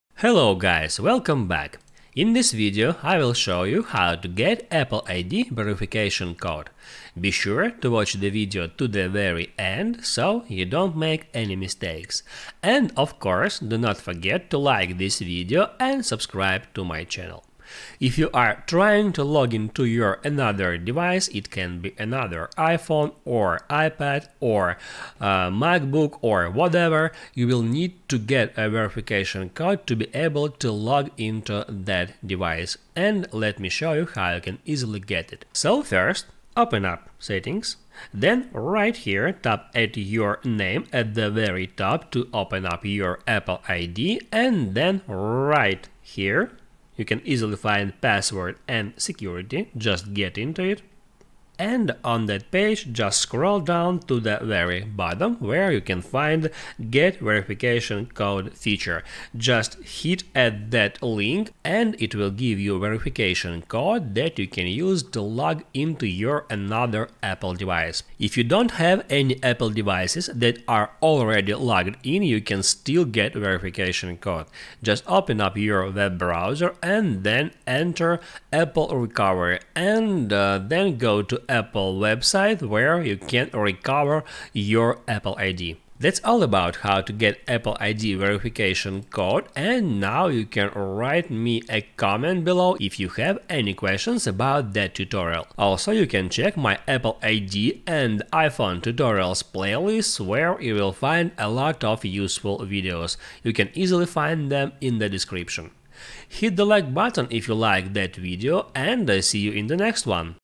How To Get Apple ID Verification Code
If you are looking for a video about how to get apple id verification code, here it is!
In this video I will show you how to get phone number verification code for apple id. Be sure to watch the video to the very end.
You will learn how to get around apple id verification code. That's easy and simple to do from your phone.